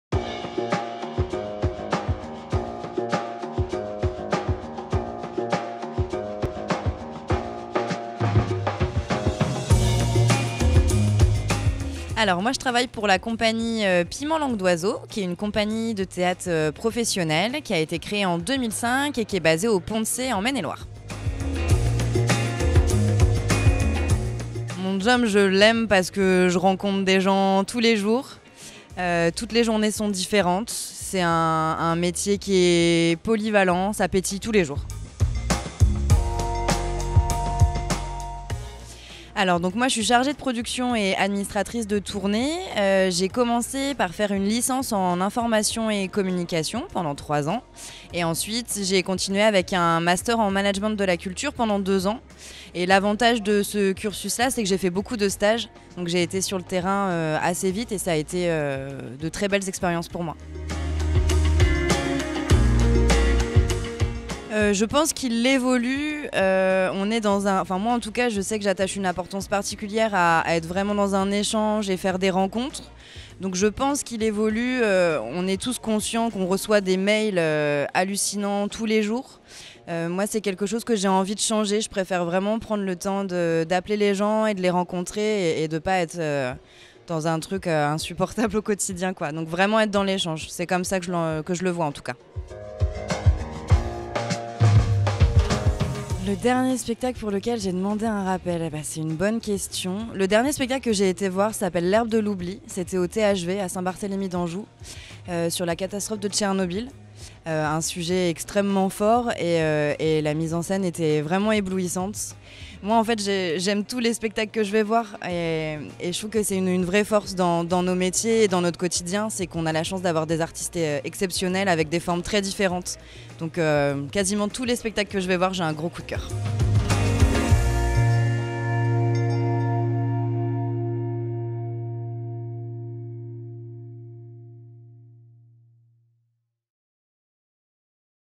Portraits de tourneurs épisode 6 : Julie Ortiz - Cie Piment Langue d'Oiseau
Dans le cadre des BIS - Biennales Internationales du Spectacle, SUN met les tourneurs sur le devant de la scène dans des interviews filmées.
Pour le sixième épisode, on reçoit Julie Ortiz, de la Cie Piment Langue d'Oiseau sur notre stand aux BIS